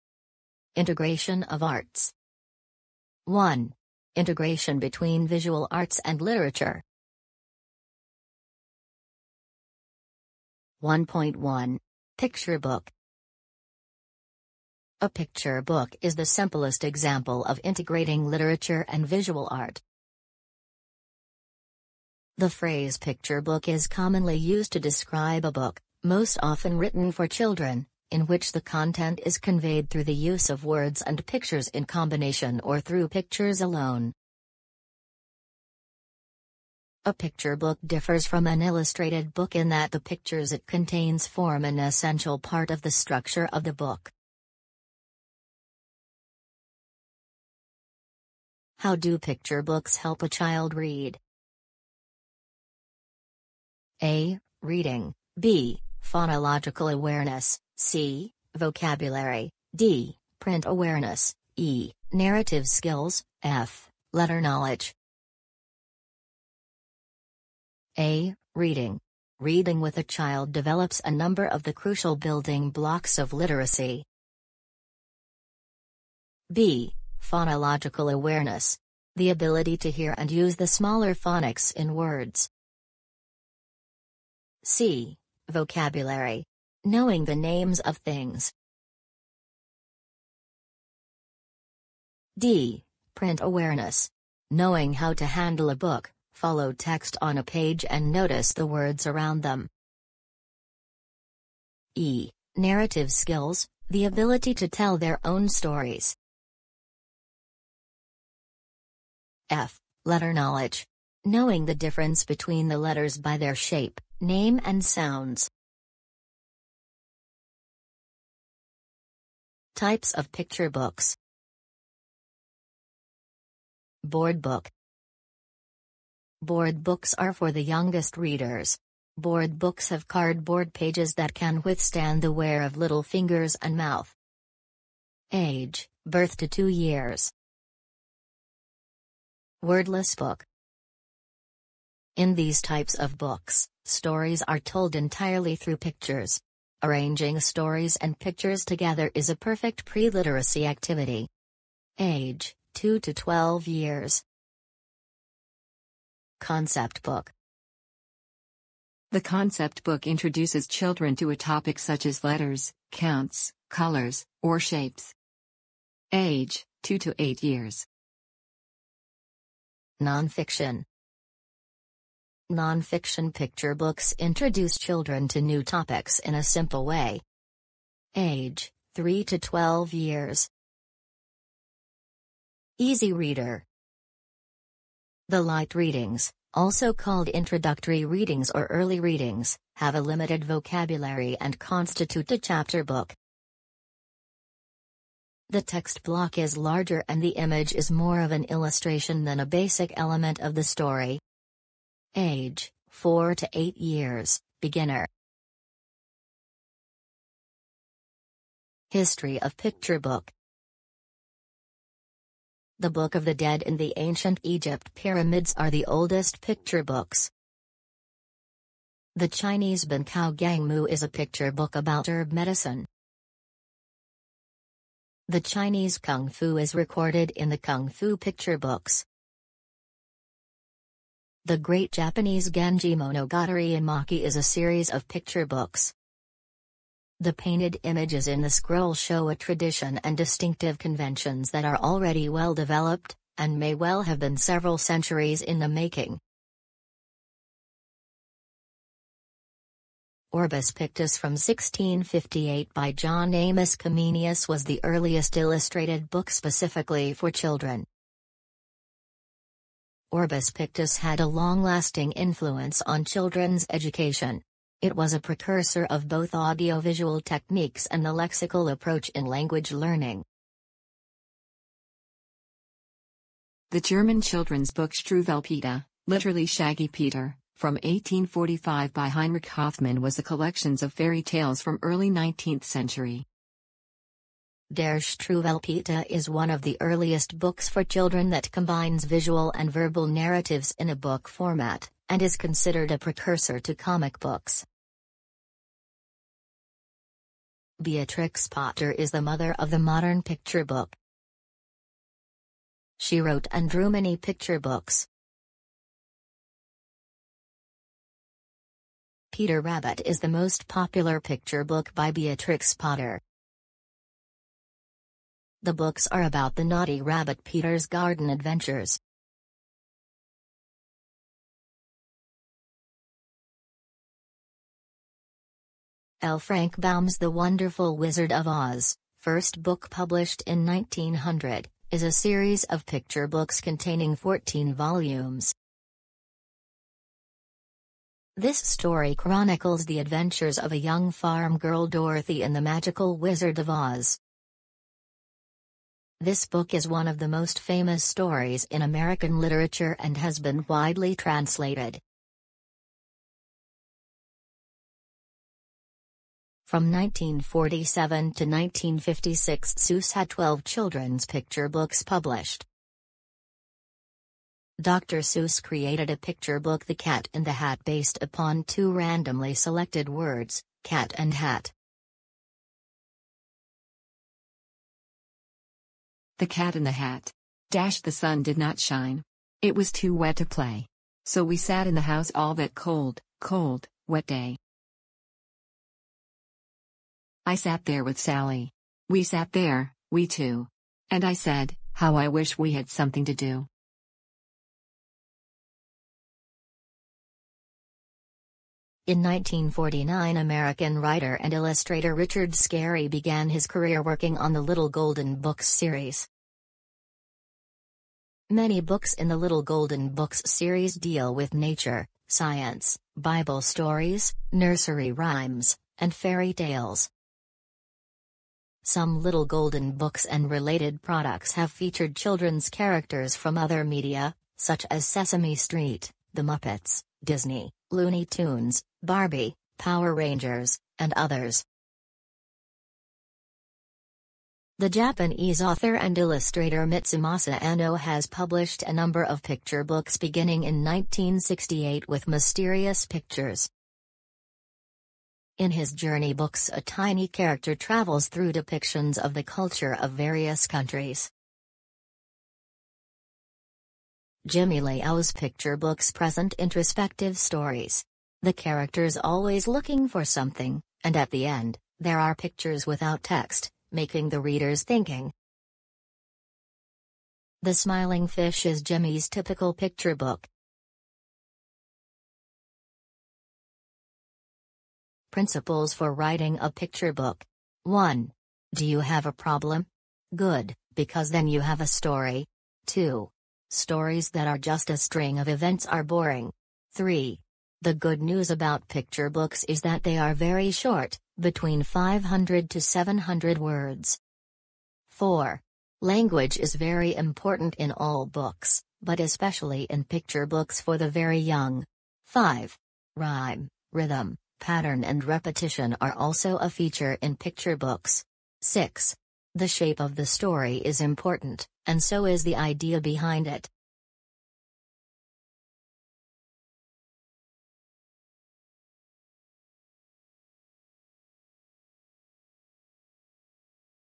Integration of arts 1.1 Picture book
The time code is attached to the directory, and you can jump to that time by pressing the time code.
(00:00:46) How do picture books help a child read?

(00:01:58) Types of Picture Books
(00:03:30) History of picture book

(00:07:54) Principles for writing a picture book